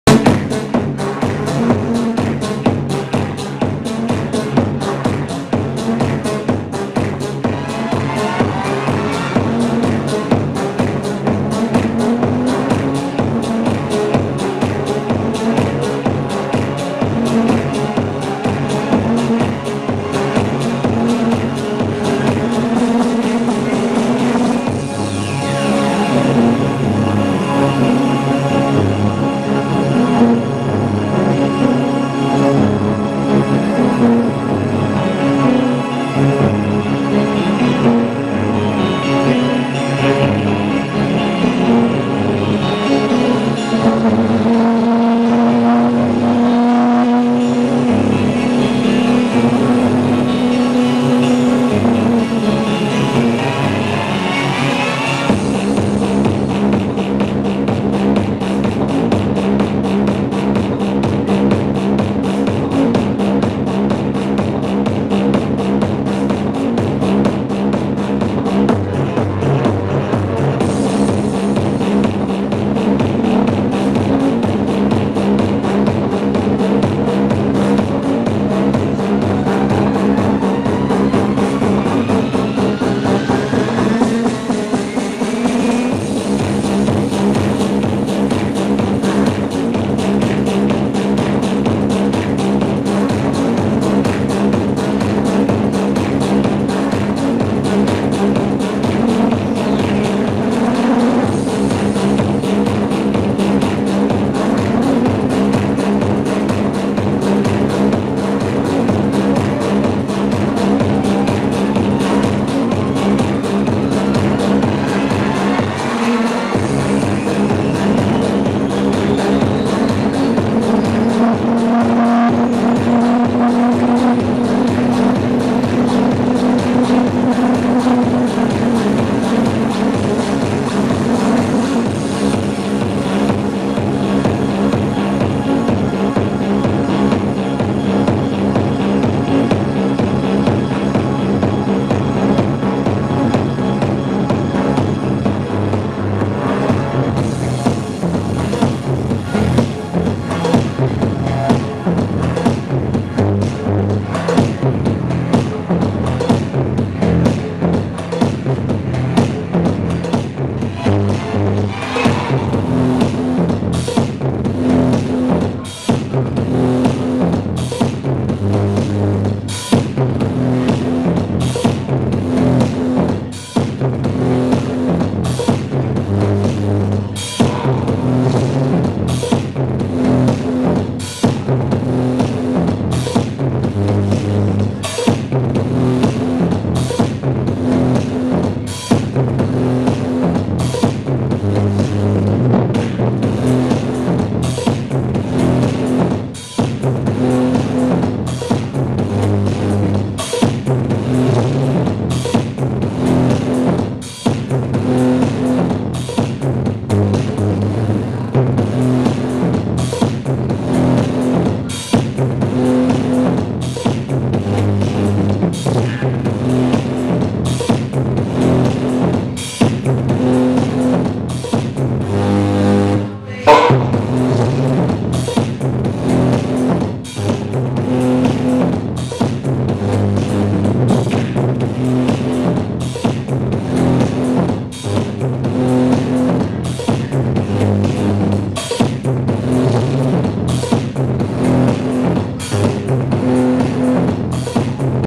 2BOY @ ever
2BOY @ Klaxons Night, Aoyama ever  - Tokyo, Japan // 09.08.10